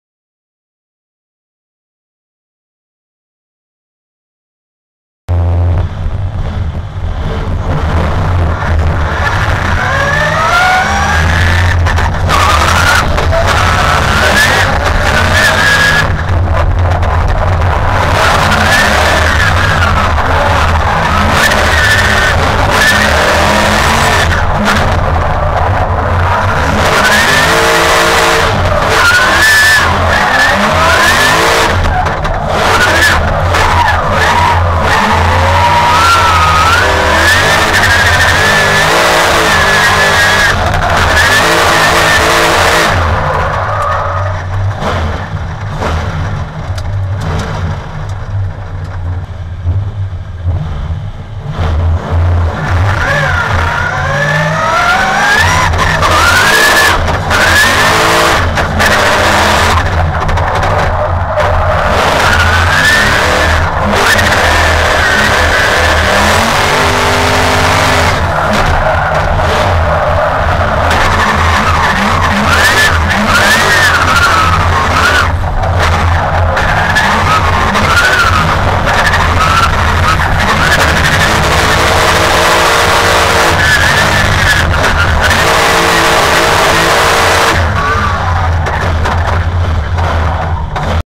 Driftworks BDC Round 4 Top 16
GoPro Vectormount footage from Round 4 of the British Drift Champion showing Phil Morrison in the V8 powered DW86 against Team Falken Driver, Matt Carter, In his 1JZ R32.